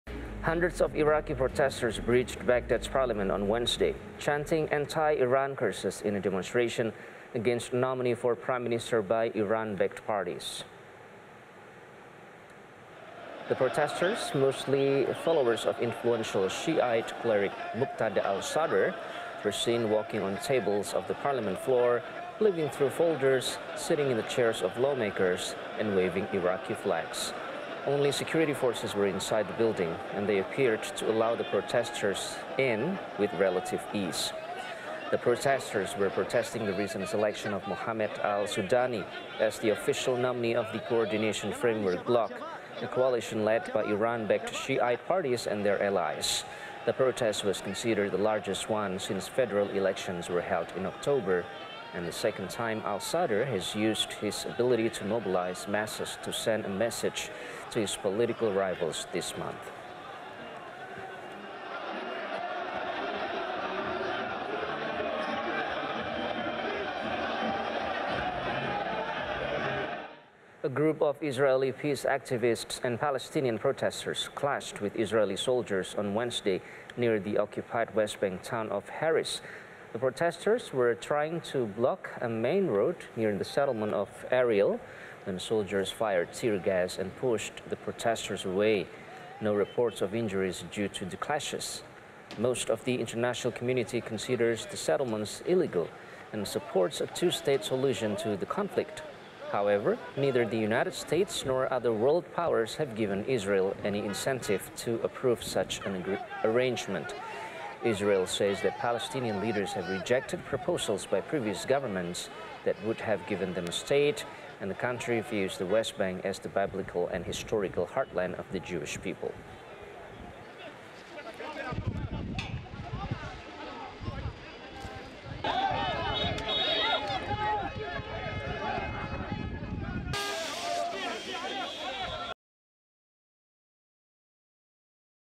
Iraqi Protesters Storm the Parliament & West Bank Clashes
Hundreds of Iraqi protesters breached Baghdad’s parliament on Wednesday, chanting anti-Iran curses in a demonstration against a nominee for Prime Minister by Iran-backed parties. Meanwhile, a group of Israeli peace activists and Palestinian protesters clashed with Israeli soldiers on Wednesday near The Occupied West Bank Town of Haris.